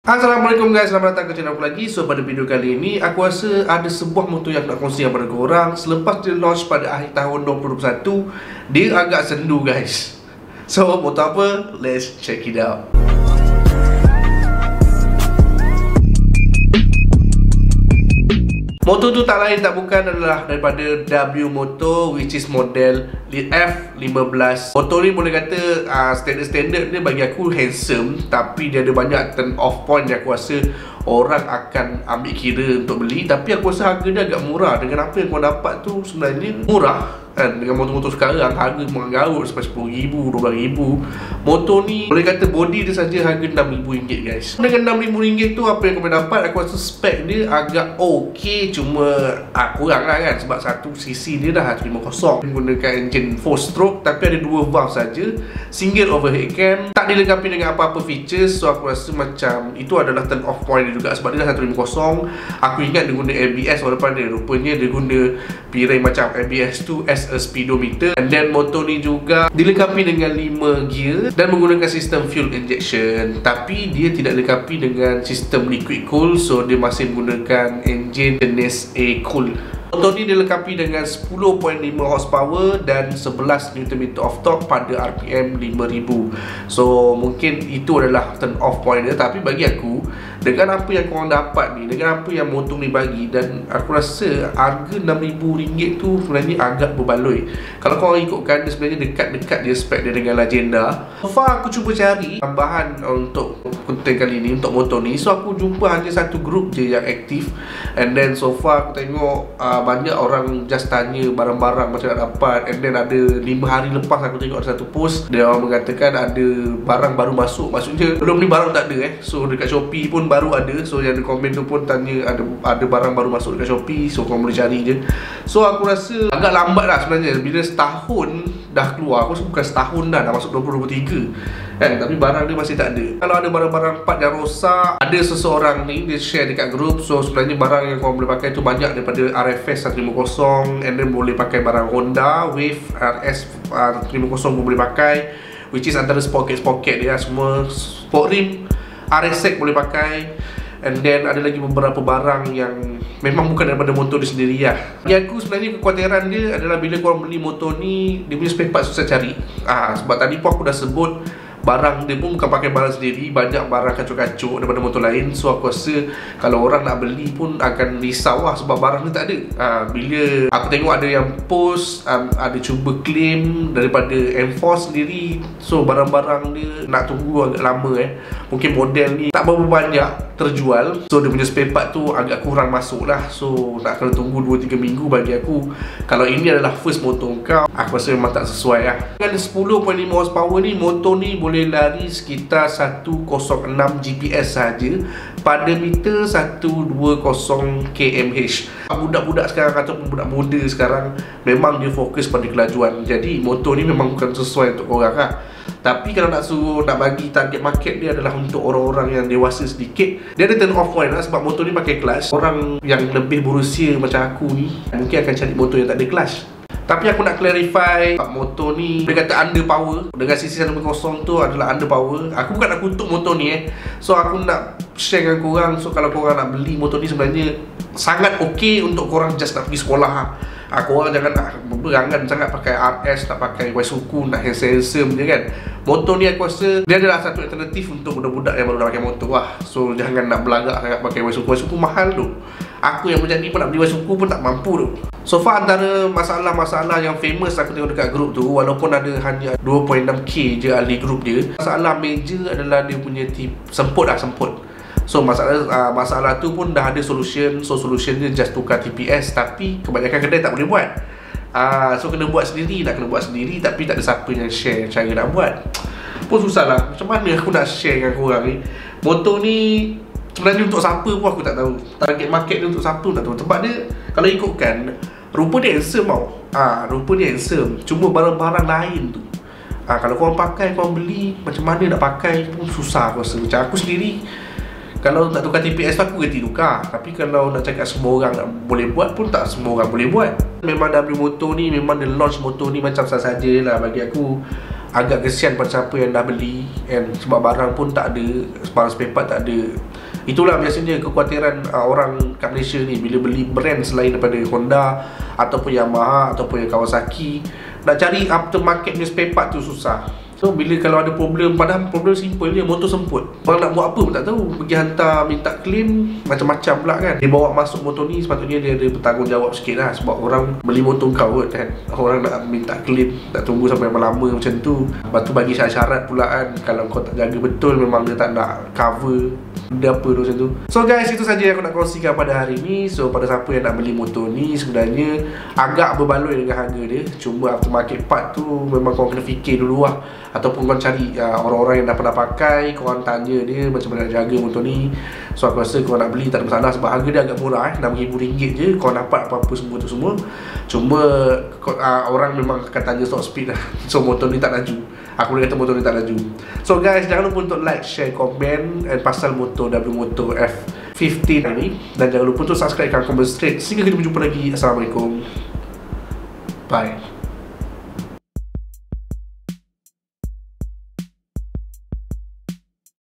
APA CERITA WMOTO F15 Sekarang
Motor ni masa launch agak hype gak tpi lepas tu senyap terus. mungkin bukan hensem yang korang cari.